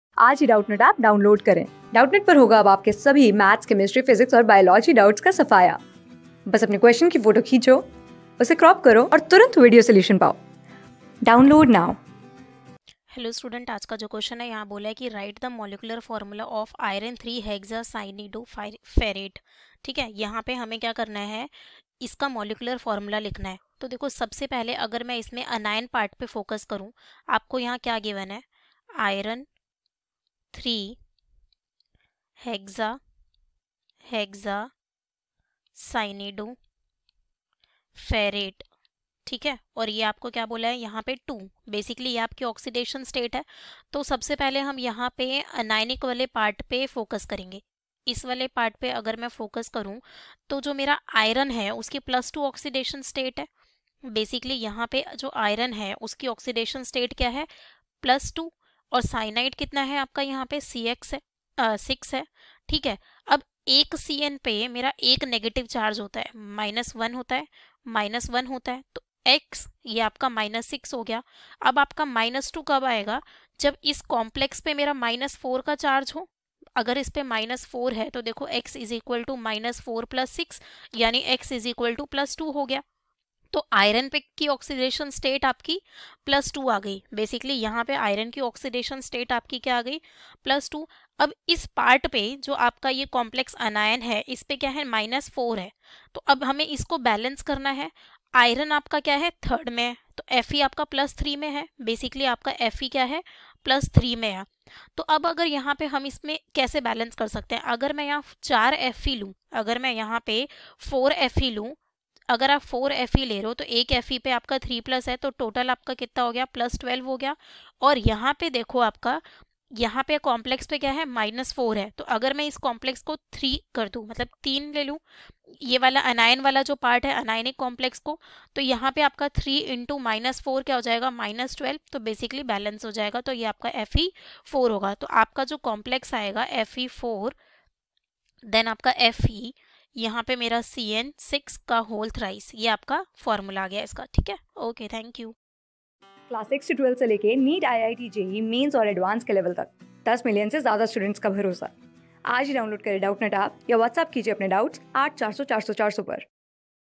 Write the molecular formula of iron (Ill) hexacyanidoferrate (II). | CLASS 11 | ORGANIC CHEMISTR...
Write the molecular formula of iron (Ill) hexacyanidoferrate (II).
Class: 11
Subject: CHEMISTRY
Chapter: ORGANIC CHEMISTRY: BASIC PRINCIPLES AND TECHNIQUES
Board:CBSE

👉 Have Any Query? Ask Us.